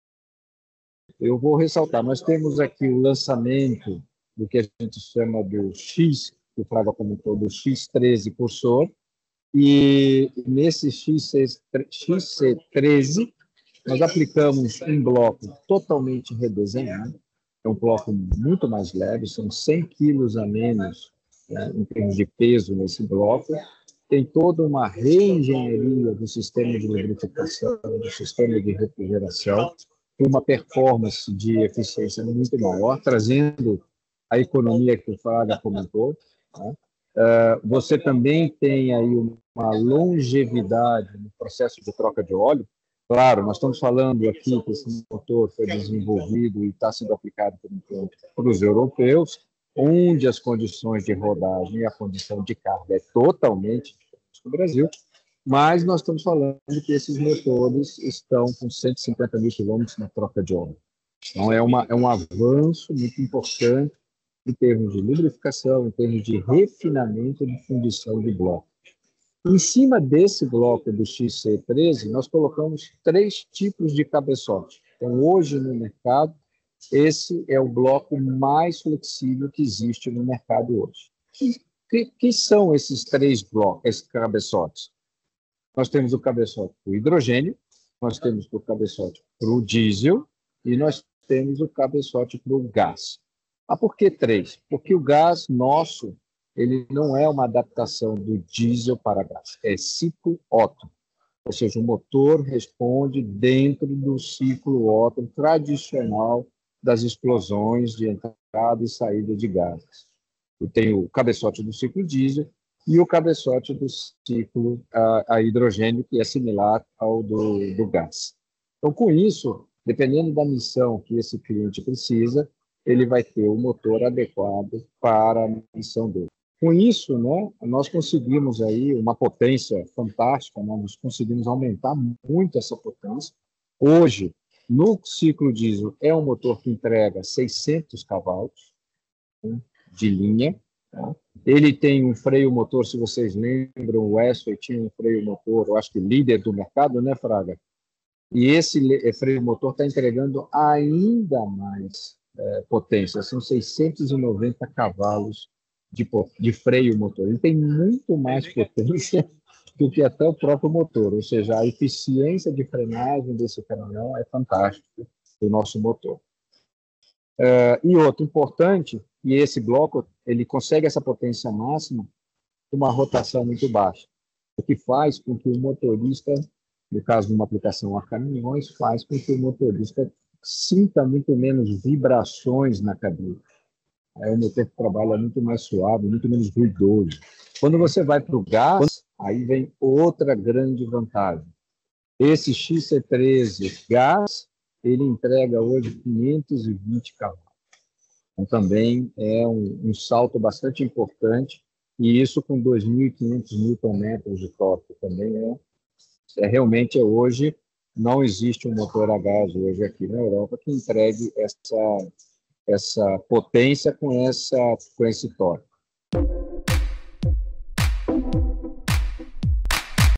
Motor XC13 reduz troca de óleo
Presidente da FPT comenta sobre novo motor que tem bloco 100 quilos mais leve